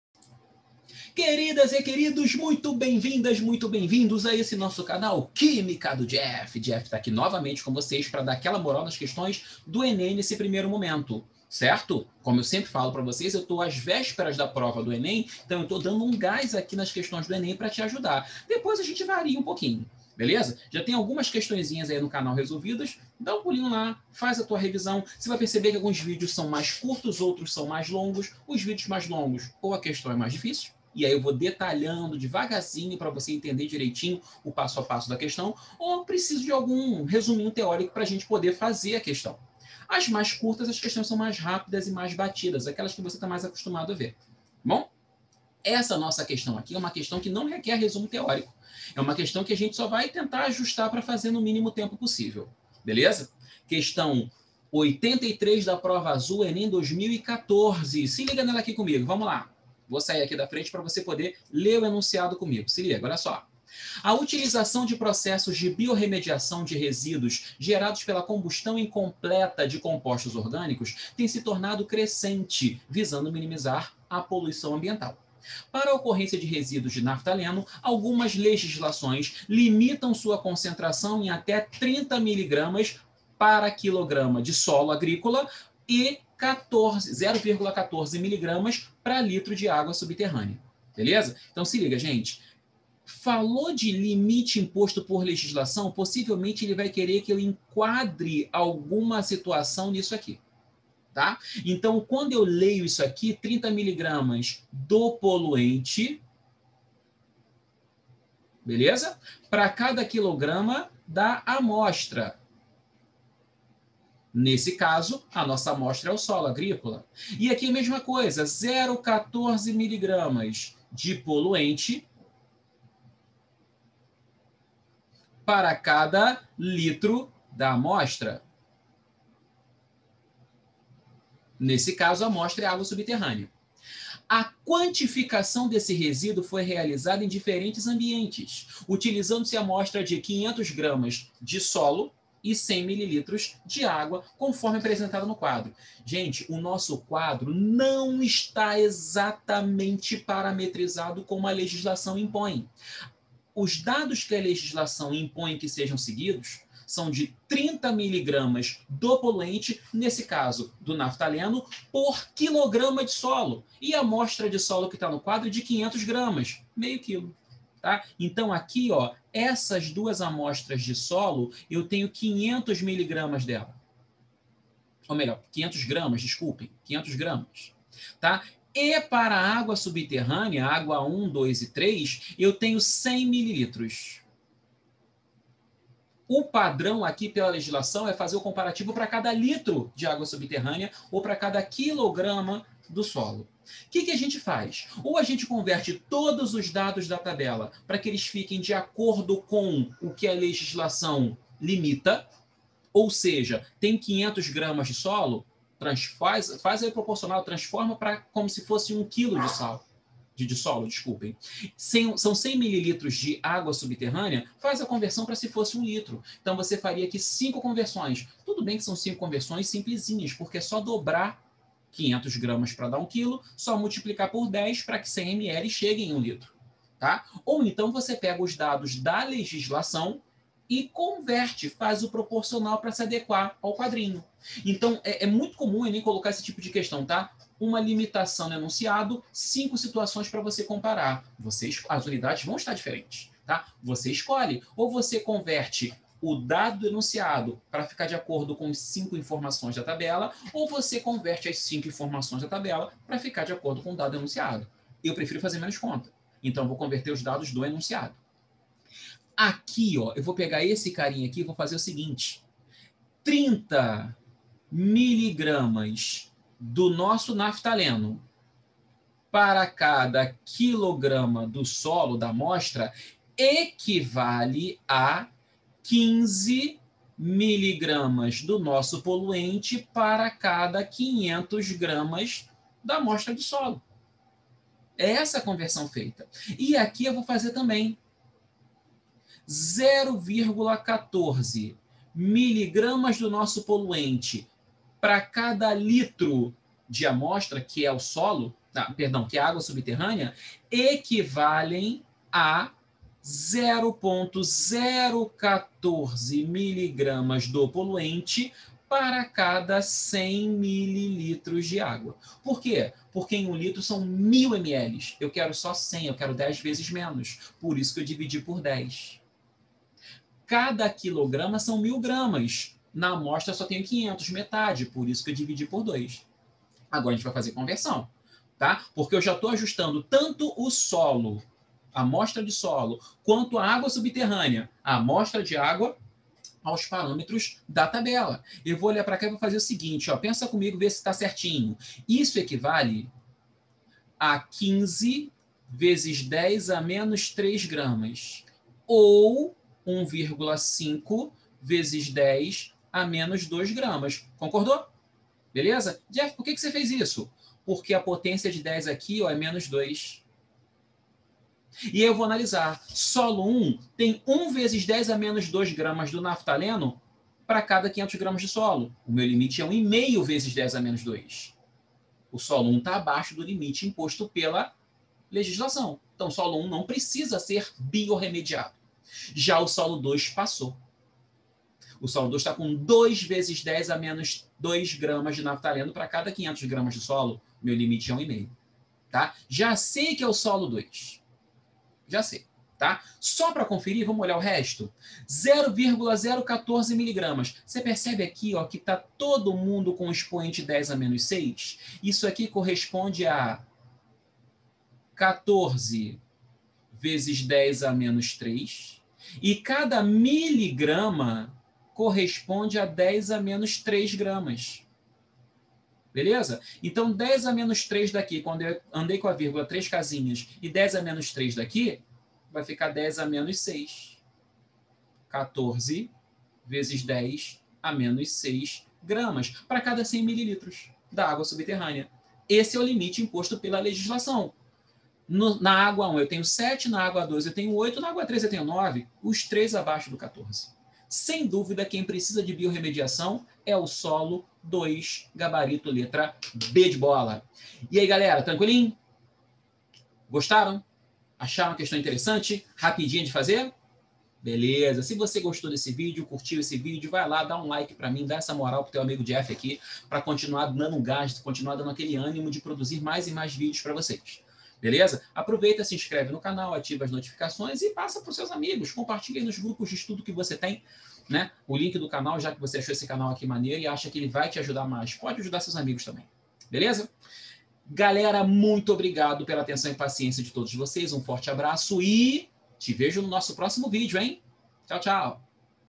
ENEM 2014: A utilização de processos de biorremediação de resíduos gerados pela combustão incompleta
Queridas e queridos,
Outro tipo comum de questão no ENEM é a que apresenta um parâmetro (poluição estipulado por legislação, por exemplo) e pede para identificarmos a amostra que requer alguma providência (biorremediação, por exemplo).
Essa questão aqui é um bom exemplo. Bora ver?

A utilização de processos de biorremediação de resíduos gerados pela combustão incompleta de compostos orgânicos tem se tornado crescente, visando minimizar a poluição ambiental. Para a ocorrência de resíduos de naftaleno, algumas legislações limitam sua concentração em até 30 mg/kg para solo agrícola e 0,14 mg/L de água subterrânea. A quantificação desse
resíduo foi realizada em diferentes ambientes, utilizando-se
amostras de 500 g de solo e 100 mL de água, conforme
apresentado no quadro.
Ambiente Resíduo de naftaleno (g)
Solo I     1,0 x 10^2
Solo II    2,0 x 10^2
Água I    7,0 x 10^6
Água II   8,0 x 10^6
Água III  9,0 x 10^6

O ambiente que necessita de biorremediação é o(a)

A solo I.
B solo II.
C água I.
D água II.
E água III.